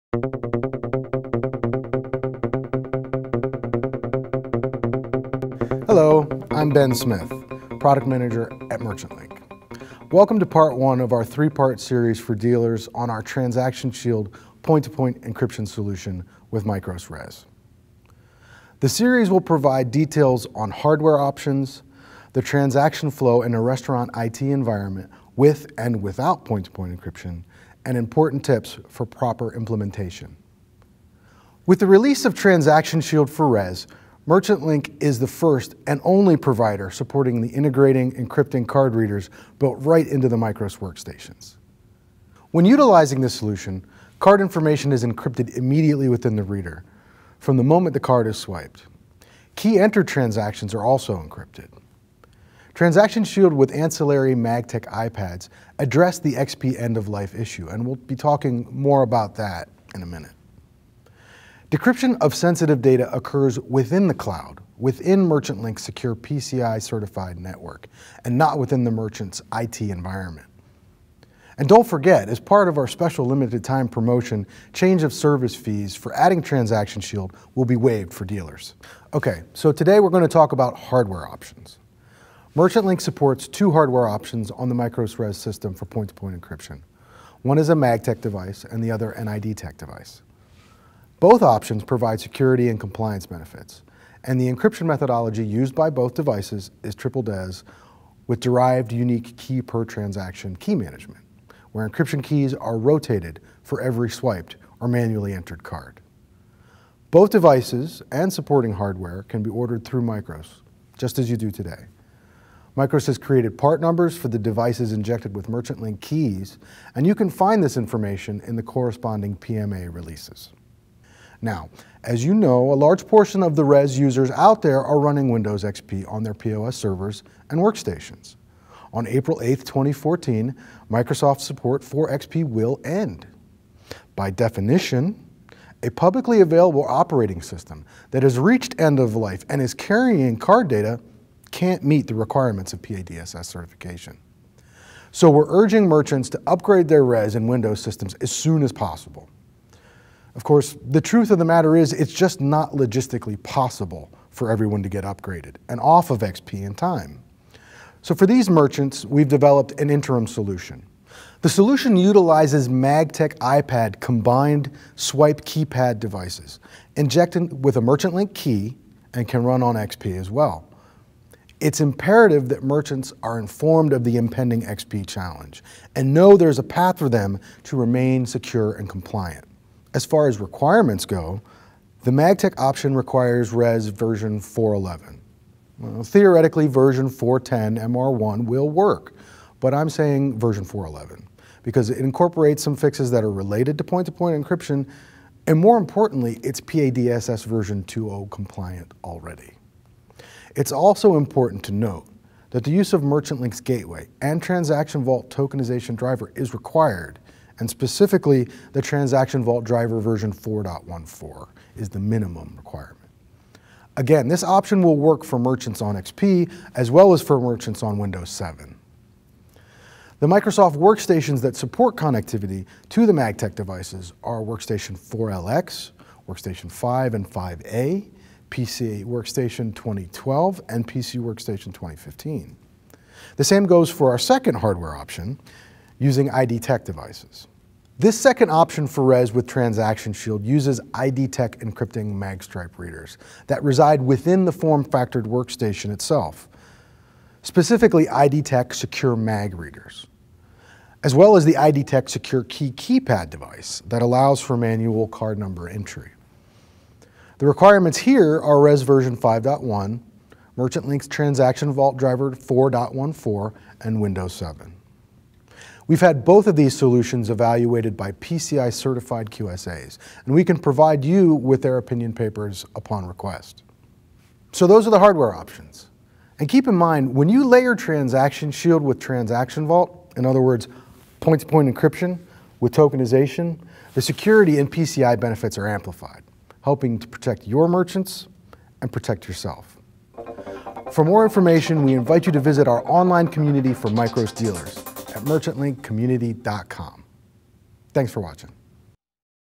TransactionShield Point-to-Point Encryption w/ MICROS RES
PART 1: Hardware Options

This is part 1 of our 3-part video series for MICROS Dealers on our TransactionShield® point-to-point encryption solution with the MICROS Restaurant Enterprise Solution, otherwise known as MICROS RES. The series provides details on hardware options, the transaction flow in a restaurant IT environment with and without point-to-point encryption and important tips for proper implementation.